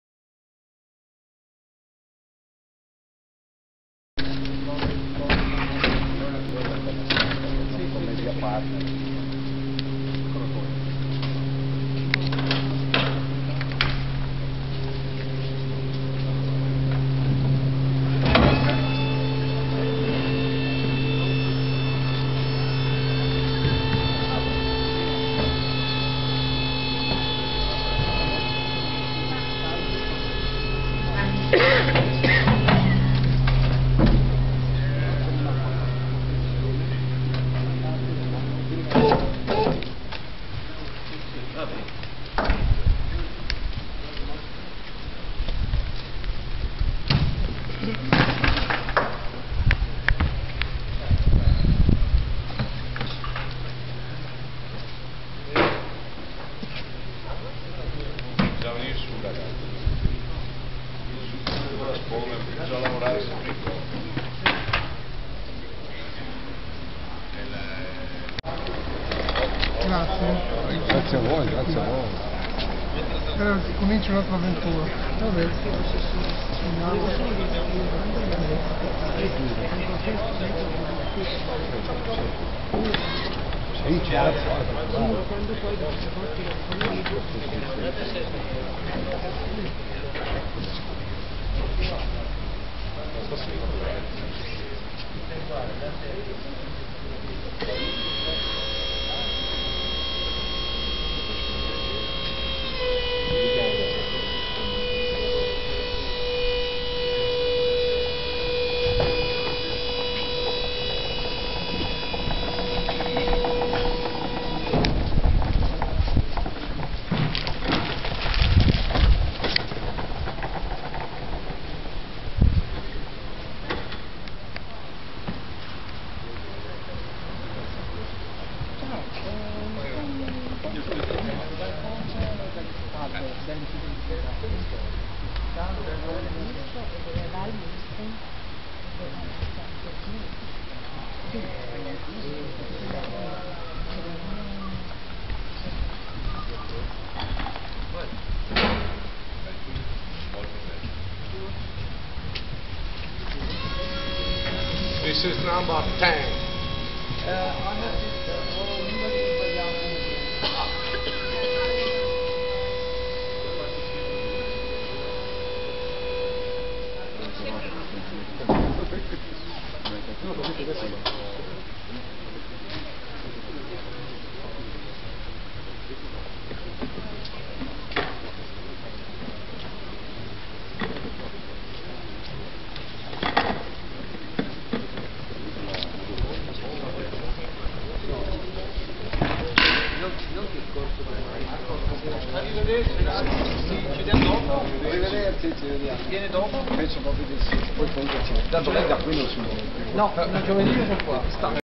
VERCELLI - Arrivano in Arca i capolavori del Guggenheim 5
Arrivano nel primo pomeriggio di sabato 25 febbraio le perle della quinta edizione della mostra Guggenheim. Sono state scaricate e portate in Arca ben venti opere provenienti dai musei newyorkesi: nel tardo pomeriggio arriverà un nuovo carico. Altri arrivi sono previsti per lunedì e martedì, mentre venerdì 2 marzo ci sarà l'avvio ufficiale della nuova avventura Guggenheim a Vercelli